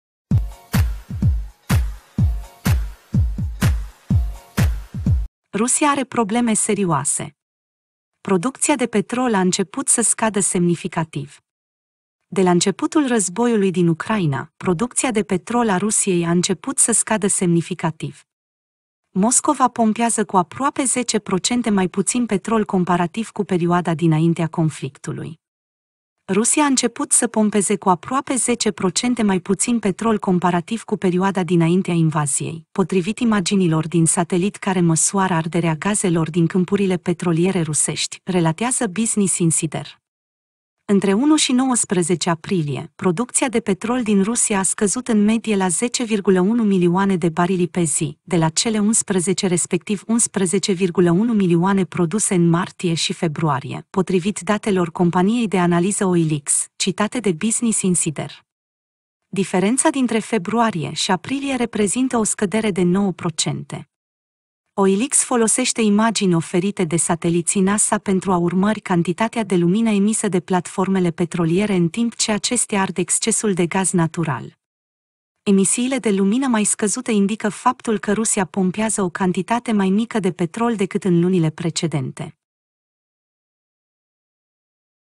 Rusia are probleme serioase! Producţia de petrol a început să scadă semnificativ
De la începutul războiului din Ucraina, producţia de petrol a Rusiei a început să scadă semnificativ. Moscova pompează cu aproape 10% mai puţin petrol comparativ cu perioada dinaintea conflictului.